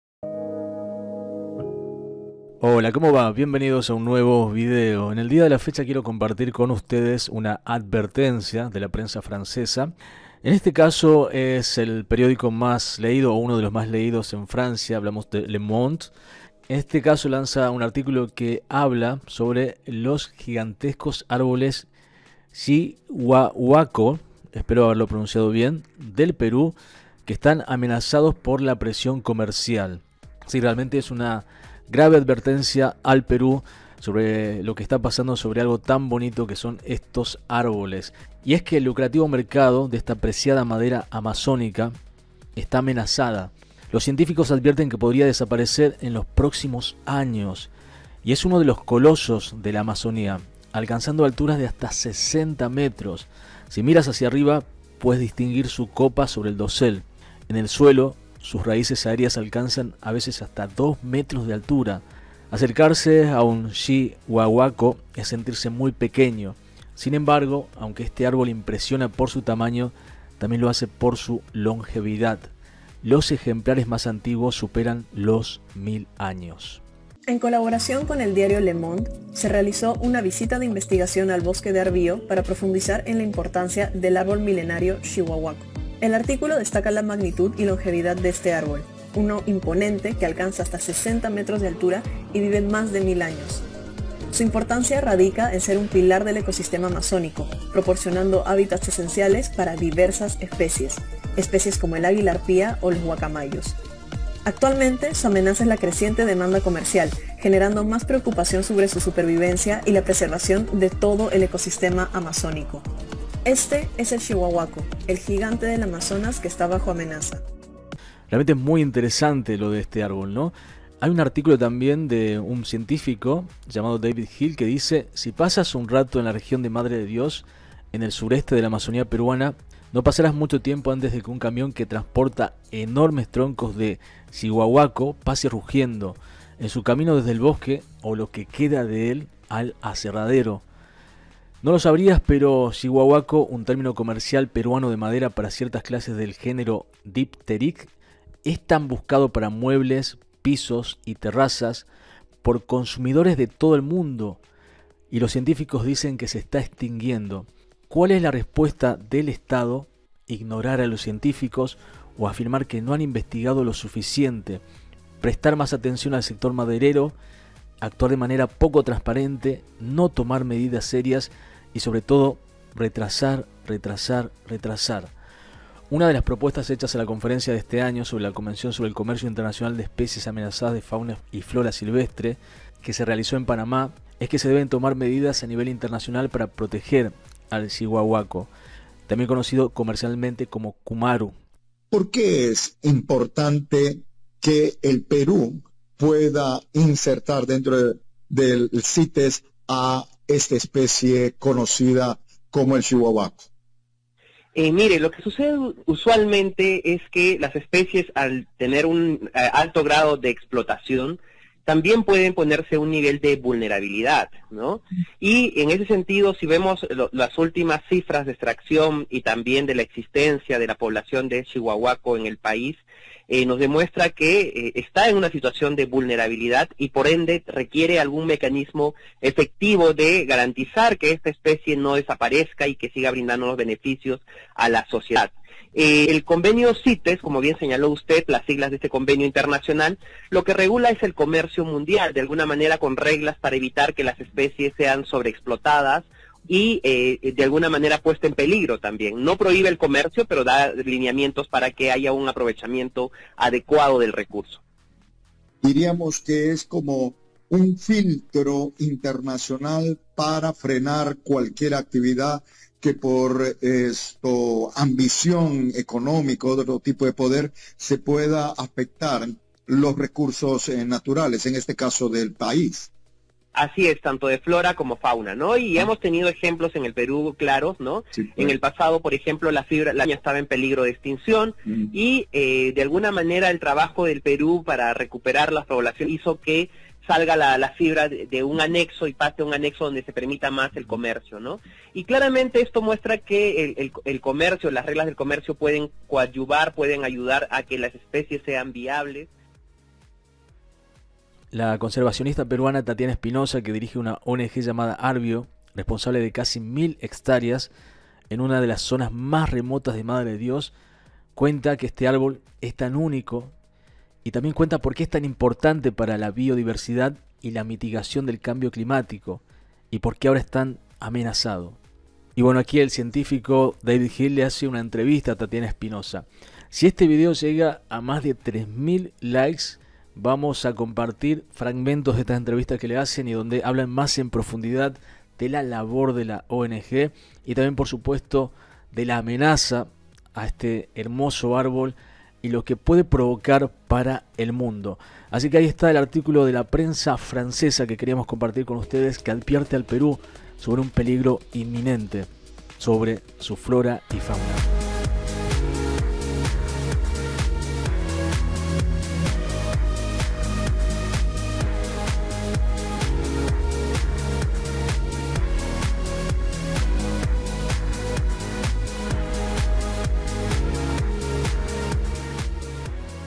PRENSA FRANCESA ADVIERTE A PERÚ SOBRE UNA AMENAZA SOBRE SU TERRITORIO
Dipteryx micrantha, conocida como shihuahuaco o cumarú, es una especie árborea de la familia Fabaceae. Es endémica de Brasil, Bolivia, Ecuador y Perú. Es un árbol emergente que puede sobrepasar los 50 m de altura.
Su madera es pesada y se caracteriza por su belleza en los acabados, de un color marrón con jaspes claros y posee un lustre moderado que tiende a elevarse. Su grano es entrecruzado y la textura es media y de corte transversal. Dentro de las propiedades físicas se puede encontrar que su densidad básica es de 0.87 g/cm³, mientras que su contracción tangencial se establece en un 9.10%, también su contracción radial rodea el 5.50%, por último, la contracción volumétrica está en un 15.00% de corte tangencial. La relación T/R es de 1.60.
Su madera está muy demandada tanto en la carpintería como en pisos de parqué, apreciándose su resistencia a ataques biológicos; y no requiere ser preservada.